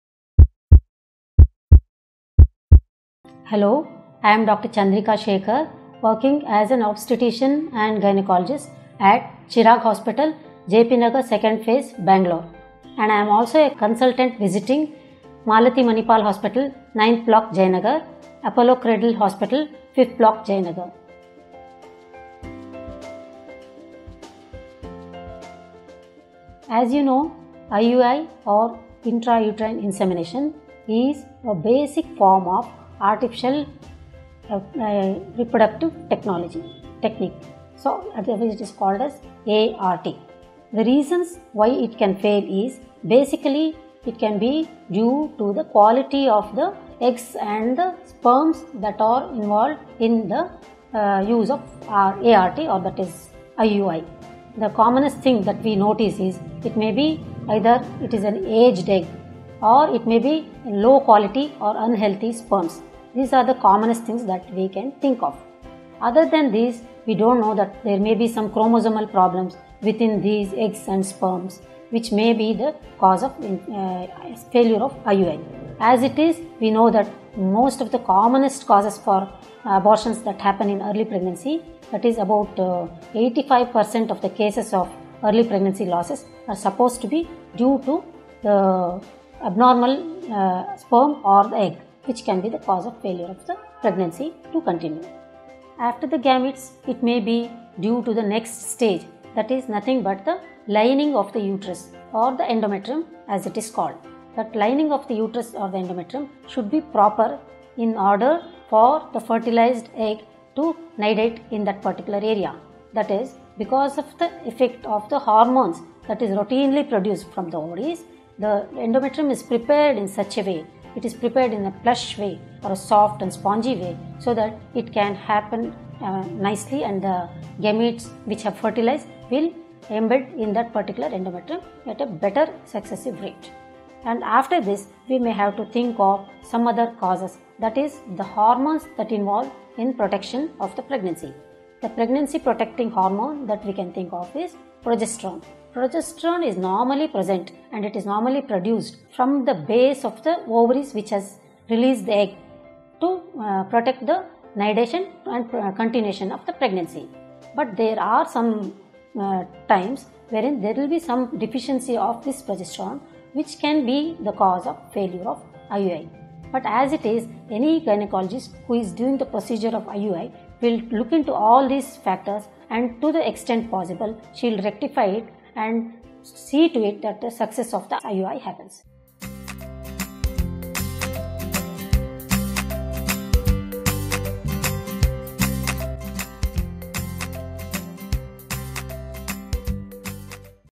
What are reasons for failure of IUI? - Dr. H S Chandrika
As you know IUI or intrauterine insemination is a basic form of artificial reproduction technique that is called as ART. The reasons why it can fail is basically it can be due to the quality of eggs and the sperms that are involved in the use of ART or that is IUI. The commonest thing that we notice is that it can be an aged egg or it can be low quality of unhealthy sperms. These are the commonest things that we can think of. Other than this there may be some chromosomal problems within these eggs and sperms which may be the cause of failure of IUI. As it is we know that most of the commonest causes for abortion that happen in early pregnancy, that is 85% of the causes of early pregnancy losses is supposed to be due to the abnormal sperm or egg, which is the cause of the failure of the pregnancy to continue. After the gametes, it can be due to the next stage, which is the lining of the uterus or the endometrium. That lining of the uterus or endometrium should be proper for the fertilized egg to nidate in that area because of the effect of the hormones, that is routinely produced in the ovaries, the endometrium is prepared in such a sway, it is pre[pared in a plush way or a soft and a spongy way so that it can happen nicely and the gametes which are fertilized will embed in the particular endometrium at a better successive rate and after this we may have to think of some other cause that is the hormones that involves in the protection of the pregnancy. The pregnancy protecting hormone that we can think of is progesterone. Progesterone is normally present and it is normally produced from the base of the ovaries which has released the egg to prevent the nidation and the continuation of the pregnancy. But there are times when there are deficiency of these progesterone, which can be the cause if the failure of IUI. But any gynecologist who is doing IUI will look into all these factors, to the extent possible, she will rectify and see to it that the success of the IUI happens.